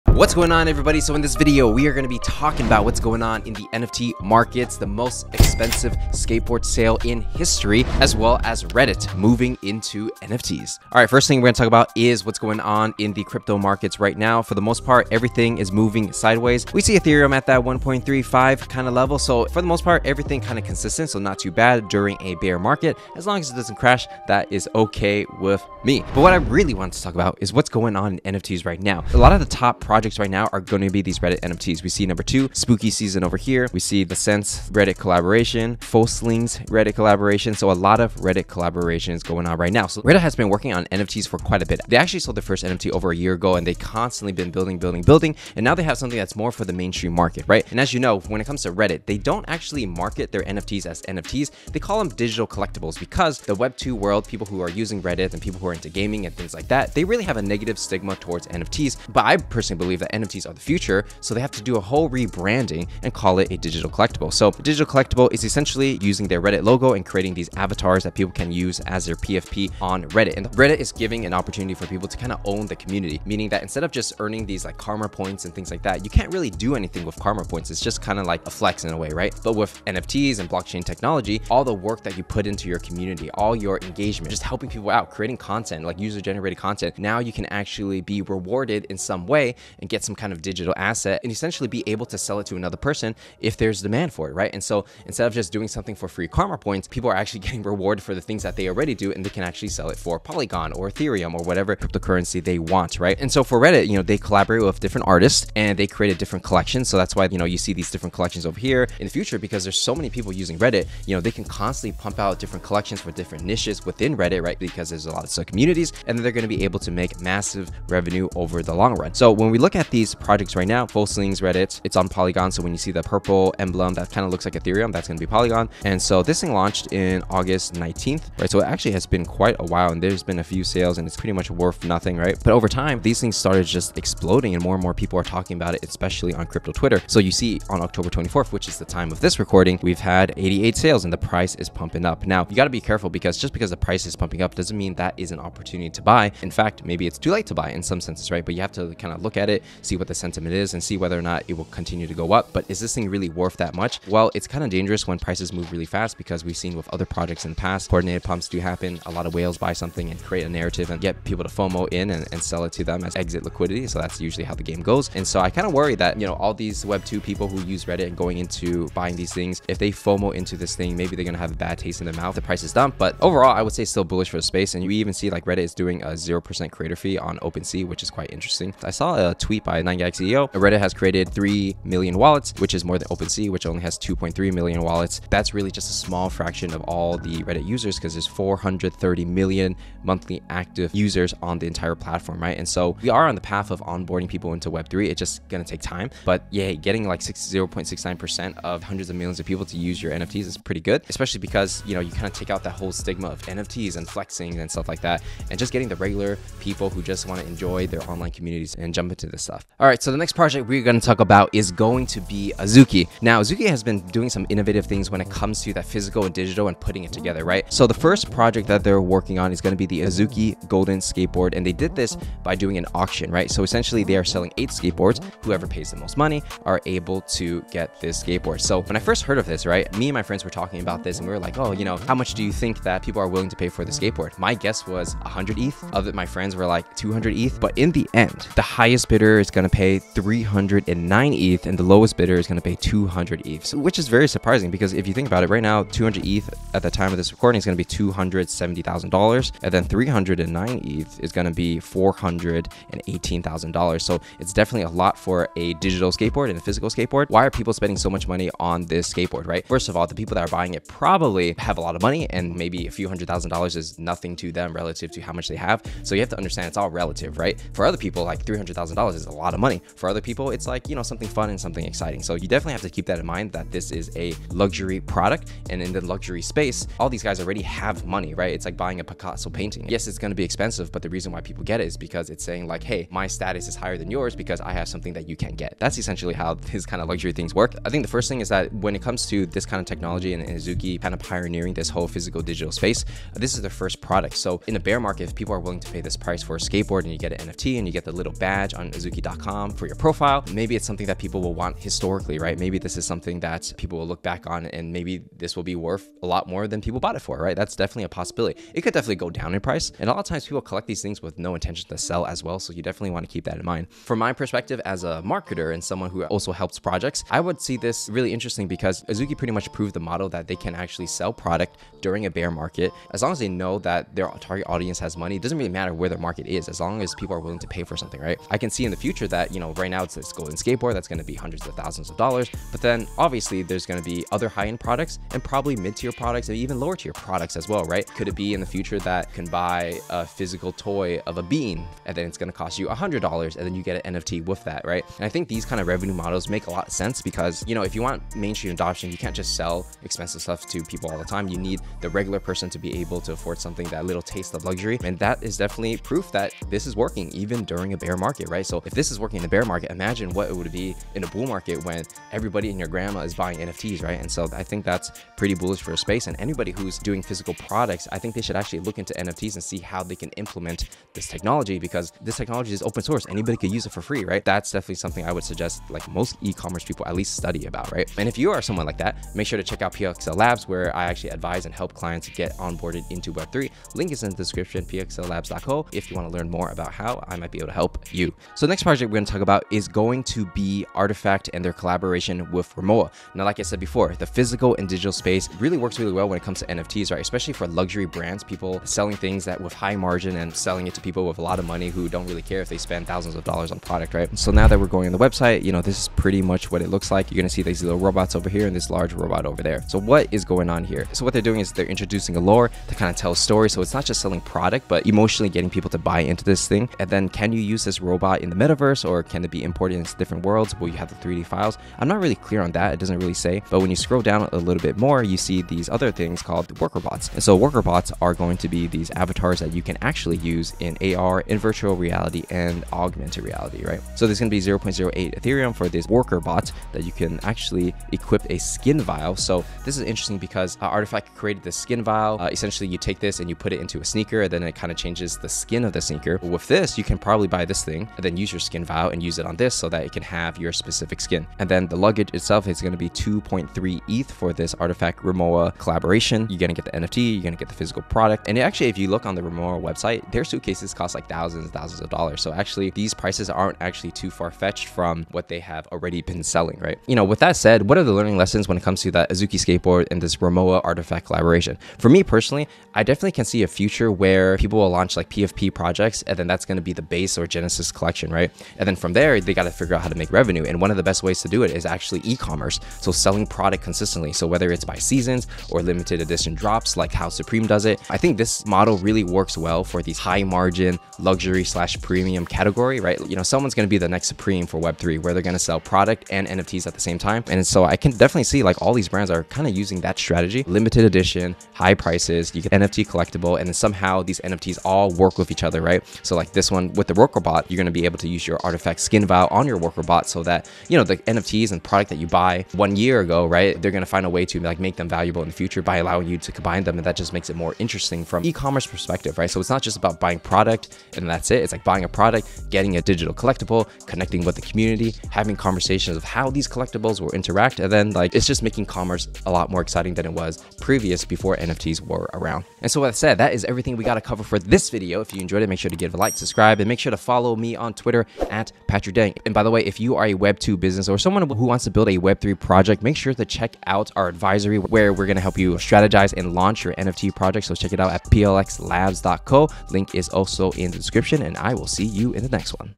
🚨 MASSIVE NFT Projects & NFT News You Missed | Reddit Avatar NFT, Azuki, CloneX Rimowa, NFTs to Buy
0:00 MASSIVE NFT Projects Update & NFT News You Missed |  Reddit Avatar NFTs, Azuki, CloneX Rimowa, NFT Drops
0:11 NFT Market Update Analysis
0:26 Reddit NFT Sales EXPLODES on Opensea
3:30 Azuki NFT Proof Of Skate Auction MASSIVE SUCCESS
6:28 RFTKT Clone X x Rimowa Upcoming NFT Drop Announcement

In this video, we'll cover today's top NFT news and updates about best upcoming NFT projects. First, we'll cover the NFT market update and crypto news. After that, we'll discuss Azuki Proof of Skate and the state of NFT flipping. Finally, we'll look at RTFKT Studios Clone X NFT collab with Rimowa.

The Parallax is a media company committed to helping you navigate the new frontier of NFTs, Web3, and crypto.